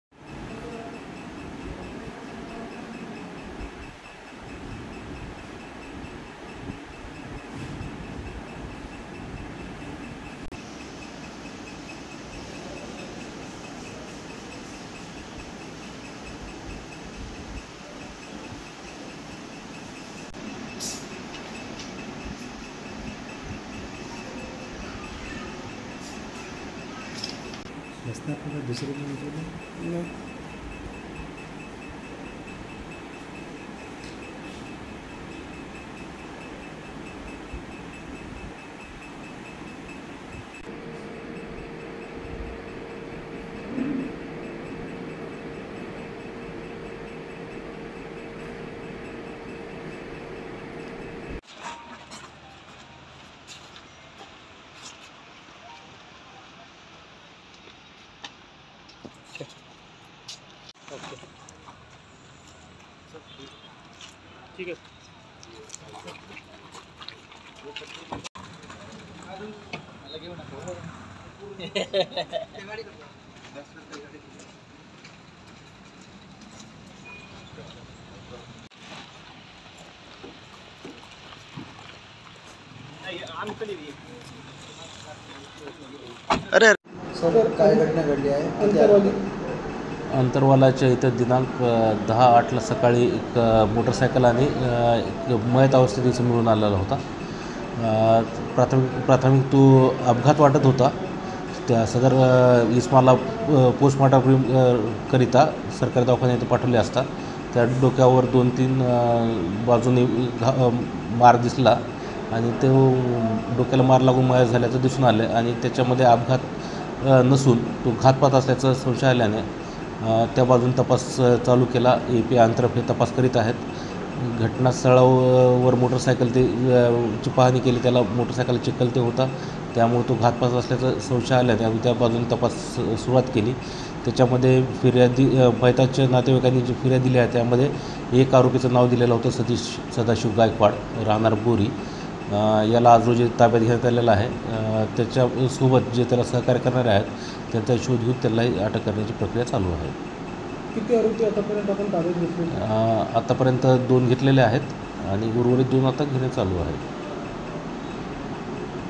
JALNA | अंतरवाला येथे इसमाचा मृत्यू अपघात नसुन खून, उत्तरीय तपासणीवरुन मारहाण केल्याचा अखेर पर्दाफाश
अंतरवाला येथे इसमाचा मृतदेह रोडवर आढळून आला होता. या प्रकरणी पोलिसांना प्रथम हा अपघात असल्याचे भासले. या वेळी उत्तरीय तपासणीसाठी रुग्णालयात दाखल केले असता या मयताच्या डोक्यात दोन्ही बाजूला मार लागल्याचे डॉक्टरांच्या वतीने पोलिसांना सांगण्यात आले. यावरून पोलिसांनी तपासाची चक्रे फिरवून यात हा अपघात नसून घातपात असल्याचे पोलिसांना आढळून आले. यावरून संशयित सतीश सदाशिव गायकवाड रा. बोरी याला ताब्यात घेतले आहे व त्याच्यासोबत जे पण त्याला या गुन्ह्यात सहकार्य करणारे आहे. त्यांचा सुद्धा शोध सुरू असून लवकरच त्यांना सुद्धा अटक केली जाणार असल्याची माहिती यावेळी पोलिसांच्या वतीने देण्यात आली आहे.

BYTE: सुरेश उनवणे, पोलीस निरीक्षक